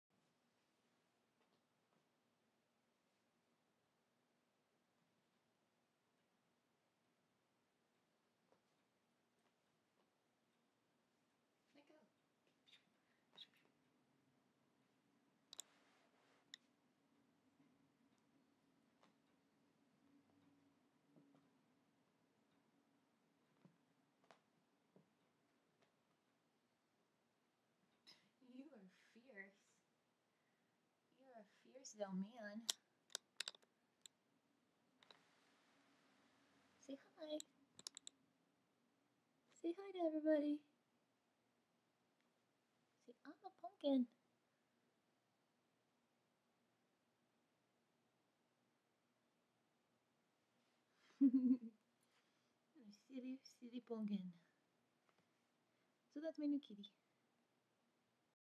My new kitten
We got a baby persian kitten tonight, May 8.  He's 9 weeks old and is a pumpkin.  I am totally in hearts with him.